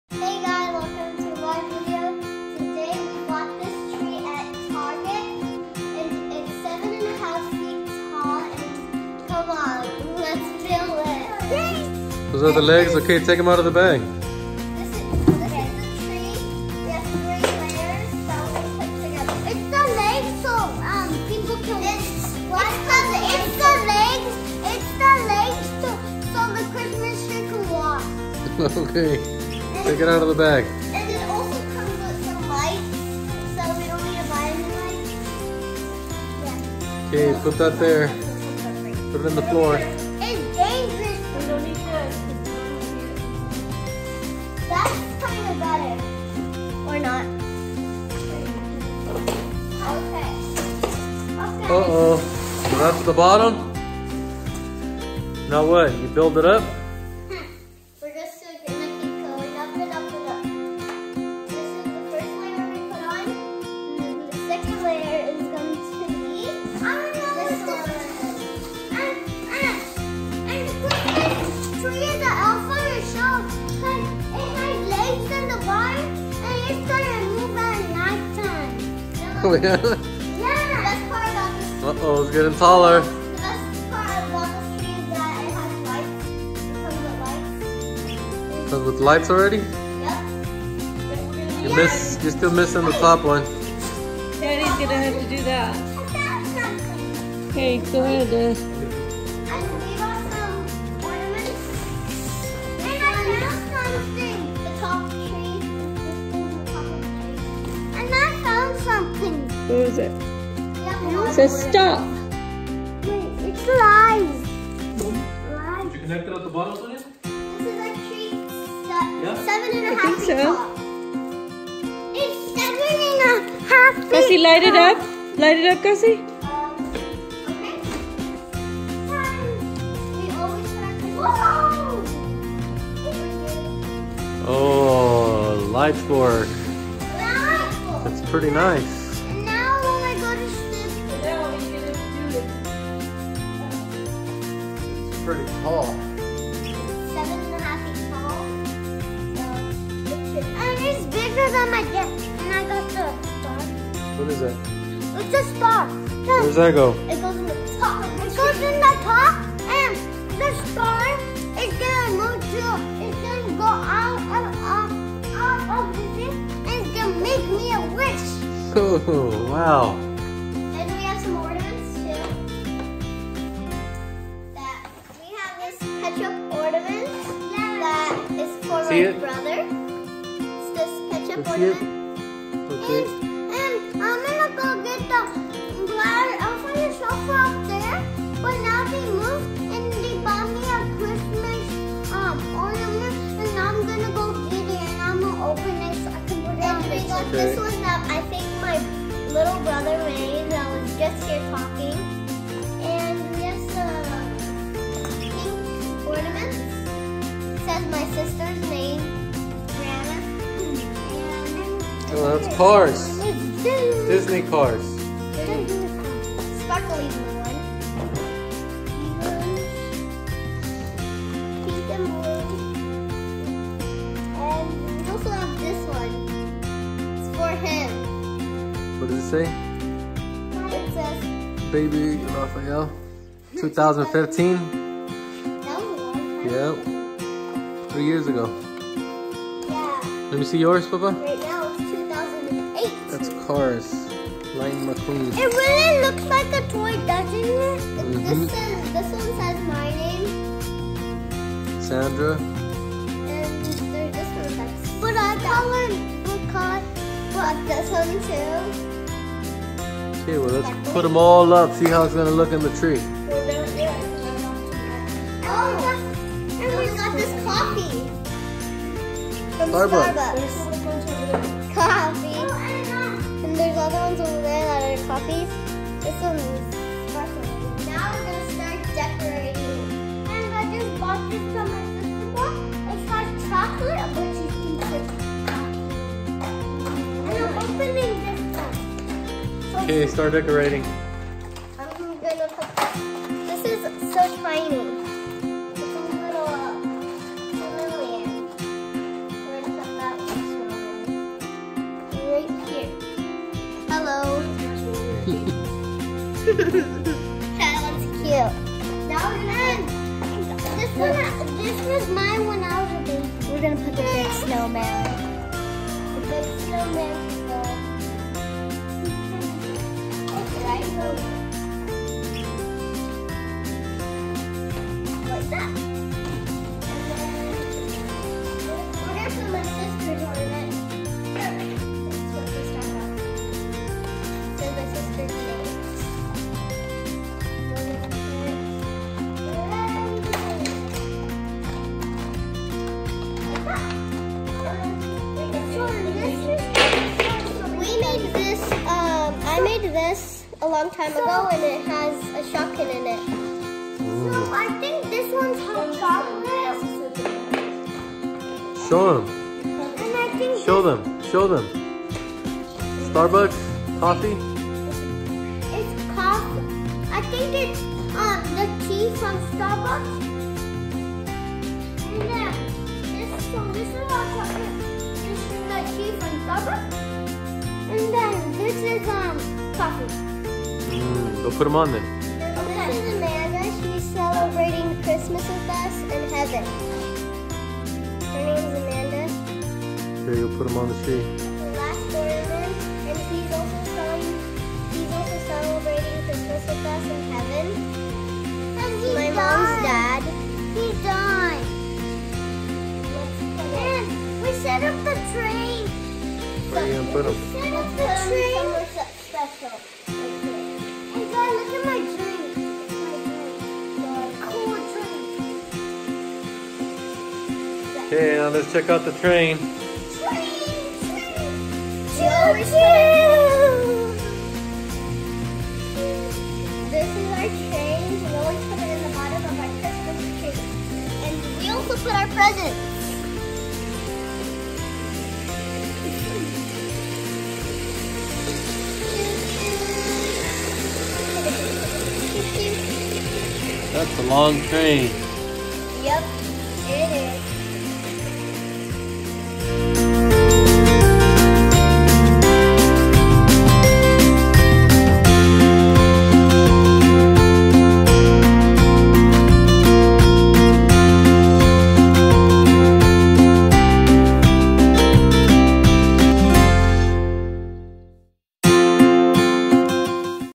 Kids setting up Wondershop Virginia pine lit Christmas Tree
kids setting up Christmas tree from wondershop Virginia pine lit tree from Target.